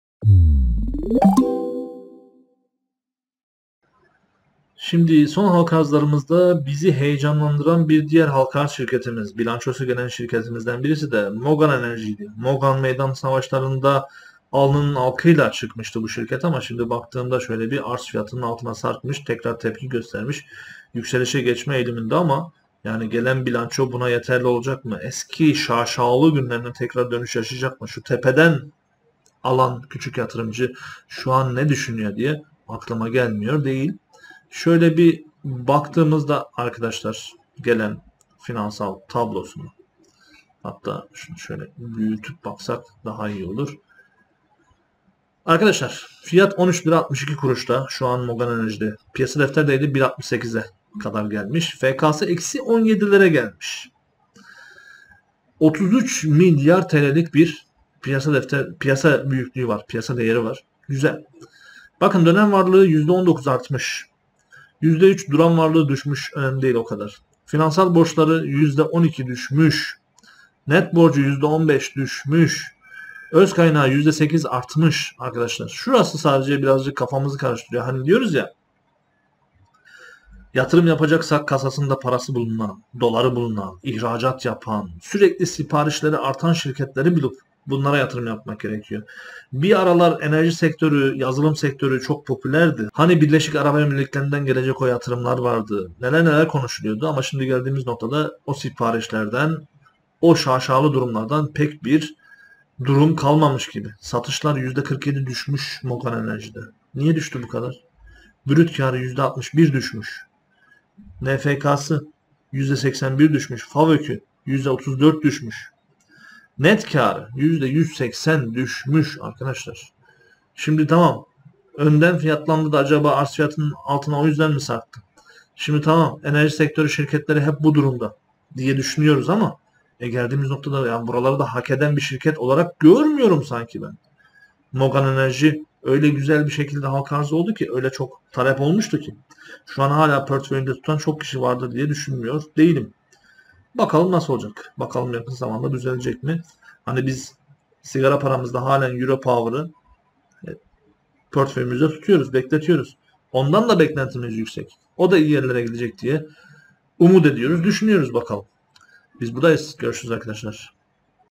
Mogan Enerji Bilançosu Geldi Halka Arz Fiyatının Altında Yakın İşlem Görüyor
Son halka arz şirketlerimizden mogan enerji bilançosu yayınlandı. Bakalım bu bilanço toparlamaya yardımcı olacak mı?